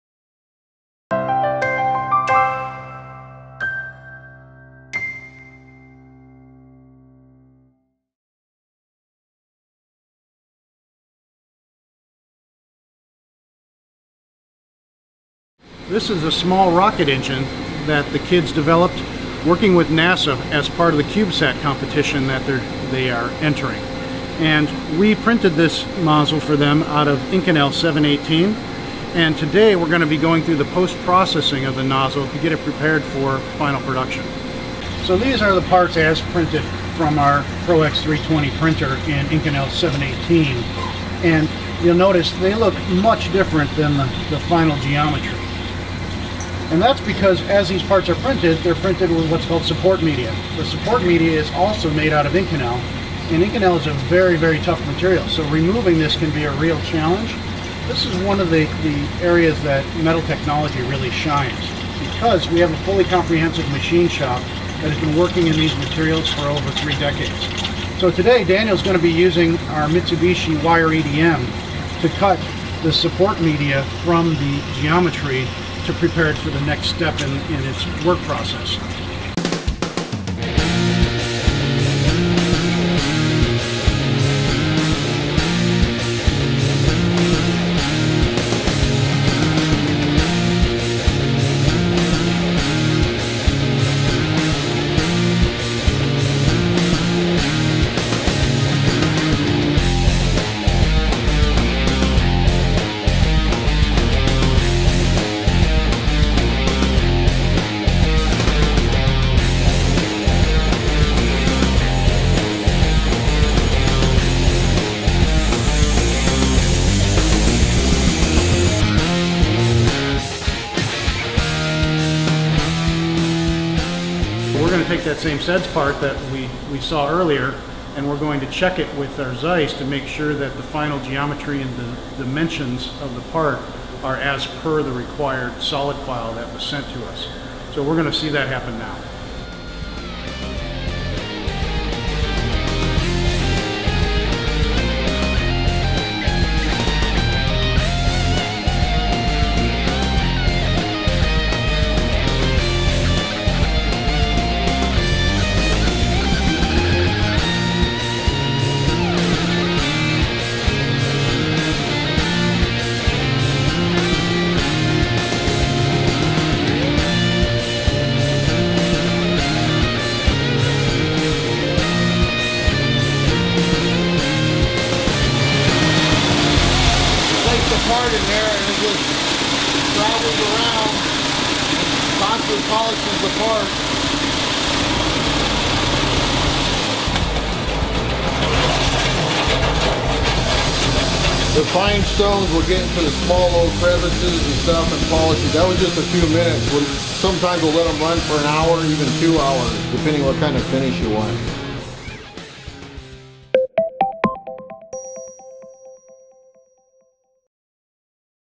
Aerospace Additive Manufacturing Post Processing | MTI & SEDS UCSD
What happens after 3D printing a space rocket engine?  Take a look at some of the post processing capabilities MTI has to offer.  The applications extend far beyond aerospace additive manufacturing. Discover why MTI is leading additive manufacturing companies with a focus on precision and dependable innovation.